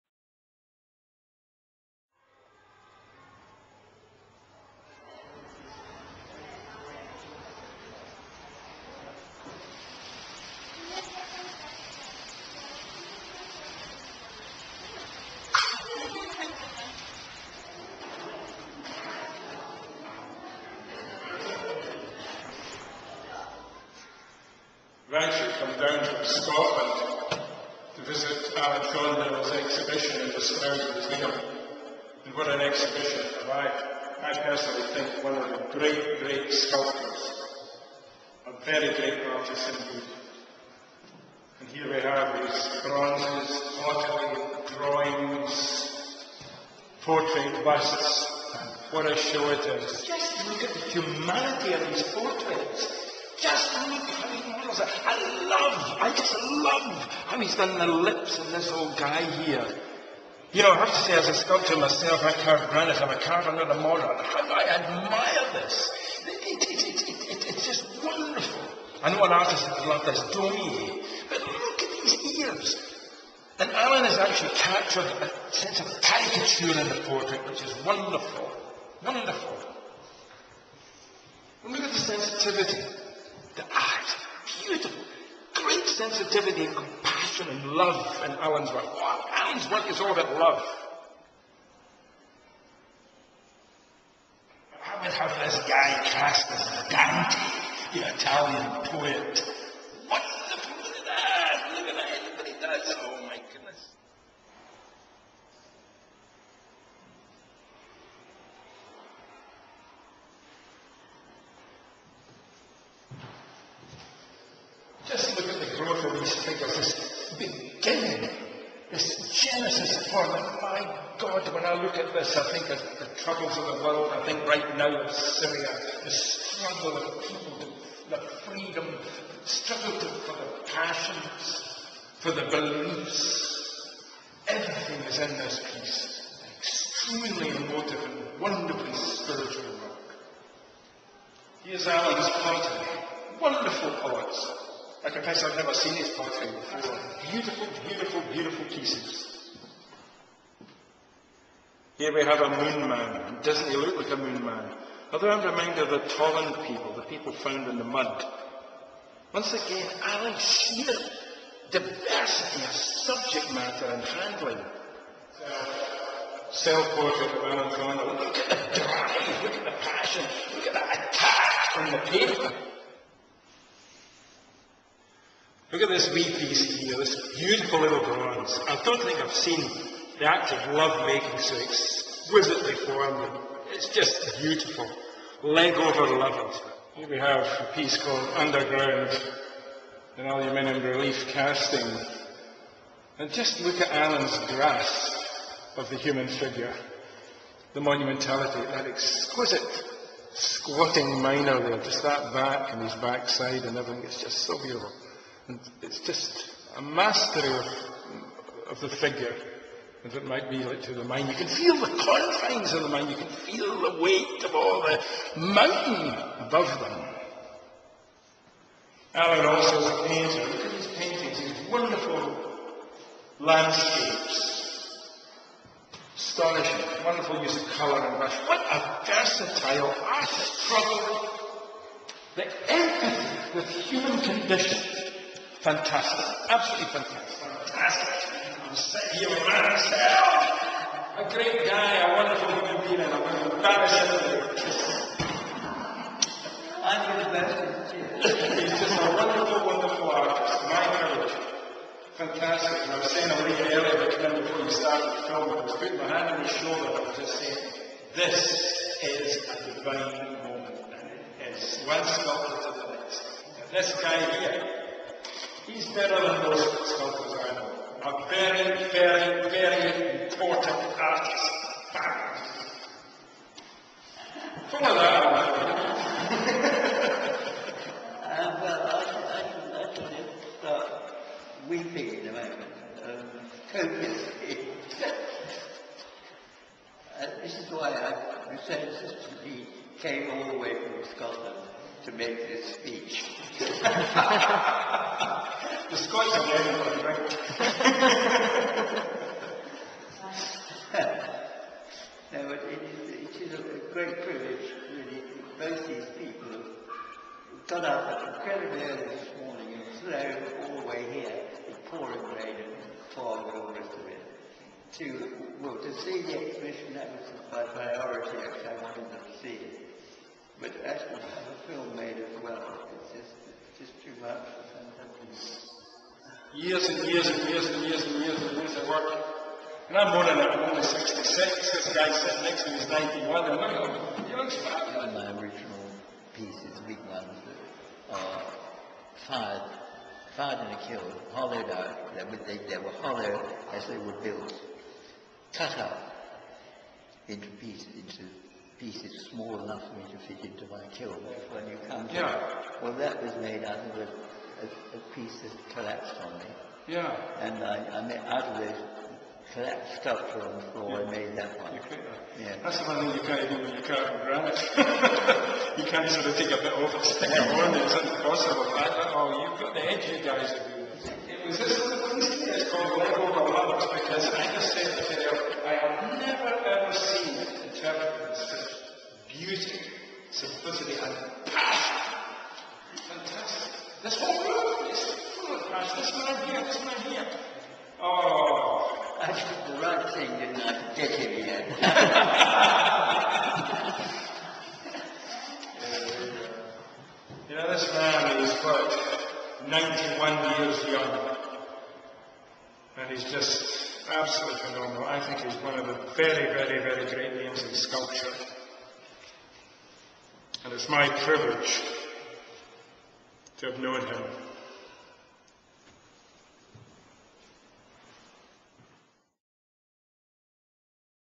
Ronald Rae visits Alan Thornhill's Exhibition at Stroud Museum
Ronald Rae enthuses about the sculpture and artworks of Alan Thornhill at his exhibition in Stroud's Museum in the Park. The film features bronze sculpture, pottery, drawings and paintings made during the lifetime of Alan Thornhill now aged 91.